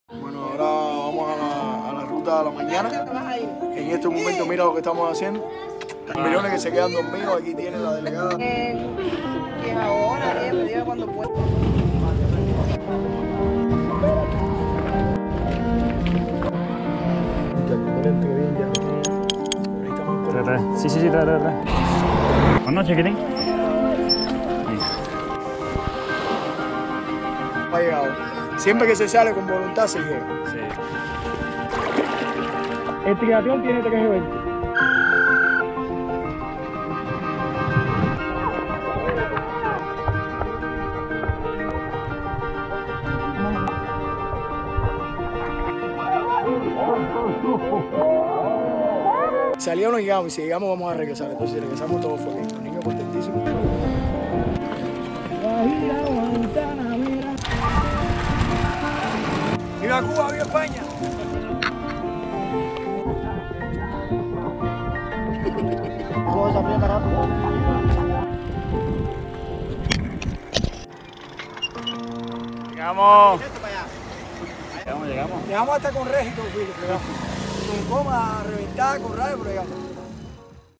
Ángeles de Trihércules | Tráiler
Como cada sábado los Ángeles de Trinidad que guía Yoelvis Pereira realizan una ruta para practicar deporte, enfrentándose a algunas dificultades por el camino. Yo estuve allí para vivirlo y poder mostrarlo.

Ángeles de Trihércules es un proyecto de cooperación del C.D. Cádiz Costa de La Luz iniciado por Ignacio A. Cano para facilitar el deporte a los niños y personas con menos recursos y ayudar a que puedan practicarlo.

Protagonizan: Ángeles de Trinidad
Narración: Yoelvis Pereira
Filmación y montaje: Ignacio A. Cano

T1E4: próximamente